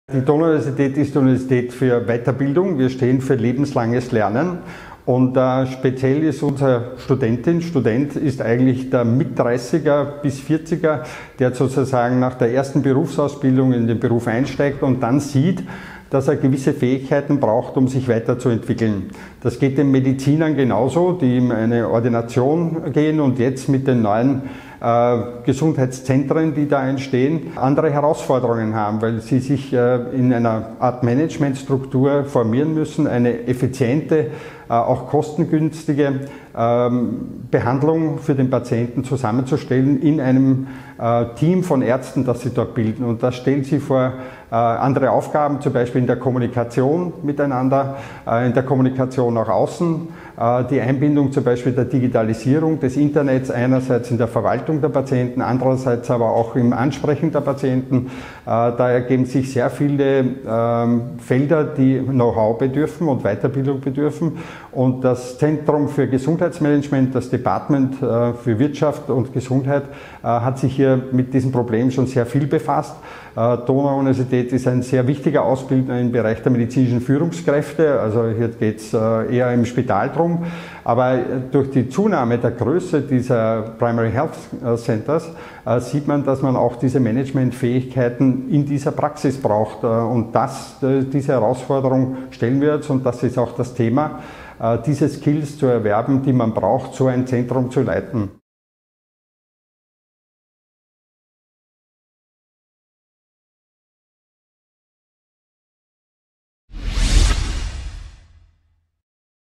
Dr. Stefan Nehrer: Fortbildungsmöglichkeiten an der der Donau-Universität Krems
📌📌📌📌📌 "𝗗𝗶𝗲 𝗗𝗼𝗻𝗮𝘂-𝗨𝗻𝗶𝘃𝗲𝗿𝘀𝗶𝘁𝗮̈𝘁 𝗶𝘀𝘁 𝗲𝗶𝗻 𝘄𝗶𝗰𝗵𝘁𝗶𝗴𝗲𝗿 𝗔𝘂𝘀𝗯𝗶𝗹𝗱𝗻𝗲𝗿 𝗶𝗺 𝗕𝗲𝗿𝗲𝗶𝗰𝗵 𝗱𝗲𝗿 𝗺𝗲𝗱𝗶𝘇𝗶𝗻𝗶𝘀𝗰𝗵𝗲𝗻 𝗙𝘂̈𝗵𝗿𝘂𝗻𝗴𝘀𝗸𝗿𝗮̈𝗳𝘁𝗲", so Univ.- Prof. Dr. Stefan Nehrer, Facharzt für Orthopädie und orthopädische Chirurgie an der Fakultät für Gesundheit und Medizin der Donau-Universität Krems, über die Fortbildungsmöglichkeiten an der der Donau-Universität Krems.

➤ 𝗦𝗔𝗩𝗘 𝗧𝗛𝗘 𝗗𝗔𝗧𝗘: 𝗗𝗶𝗲 𝗣𝗥𝗔𝗘𝗩𝗘𝗡𝗜𝗥𝗘 𝗚𝗲𝘀𝘂𝗻𝗱𝗵𝗲𝗶𝘁𝘀𝘁𝗮𝗴𝗲 𝟮𝟬𝟮𝟭 𝗳𝗶𝗻𝗱𝗲𝗻 𝗮𝗺 𝗠𝗶𝘁𝘁𝘄𝗼𝗰𝗵 𝟭𝟵. – 𝗙𝗿𝗲𝗶𝘁𝗮𝗴 𝟮𝟭. 𝗠𝗮𝗶 𝟮𝟬𝟮𝟭, 𝗶𝗻 𝘂𝗻𝗱 𝗮𝘂𝘀 𝗱𝗲𝗺 𝗦𝘁𝗶𝗳𝘁 𝗦𝗲𝗶𝘁𝗲𝗻𝘀𝘁𝗲𝘁𝘁𝗲𝗻 𝘀𝘁𝗮𝘁𝘁.